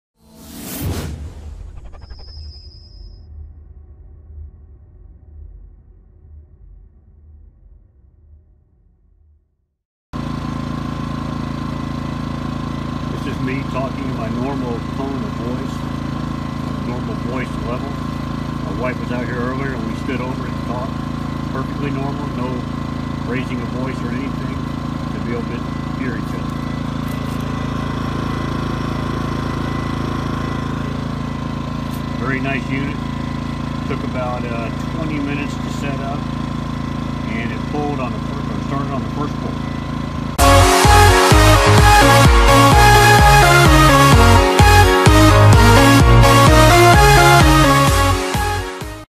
Review Generac 7117 GP2200i 2200 Watt Portable Inverter Generator 2019
*A little about Generac 7117 GP2200i 2200 Watt Portable Inverter Generator  *

Generac's GP2200i inverter generator provides outdoor enthusiasts with a portable power solution designed for a variety of open-air festivities . From weekends in the woods to outdoor cookouts and get-togethers, the GP2200i provides the stable power you need without disturbing neighbors or surrounding wildlife. Using TruePower Technology, the GP2200i produces the clean power required by a variety of sensitive electronics, tools and appliances. Compact and fully-enclosed, its lightweight design makes transportation easy, and its Economy mode feature also reduces fuel consumption rates and noise emissions to operate quietly for extended periods of time. Whether you're tailgating for the big game or simply want to enhance your campsite experience without disrupting the environment around you, Generac's GP2200i inverter generator is right for you. The parallel capability allows you to connect two units together to power your RV air conditioner, lights, cell phone (via USB port) and other portable appliances at the same time. 2 years limited warranty.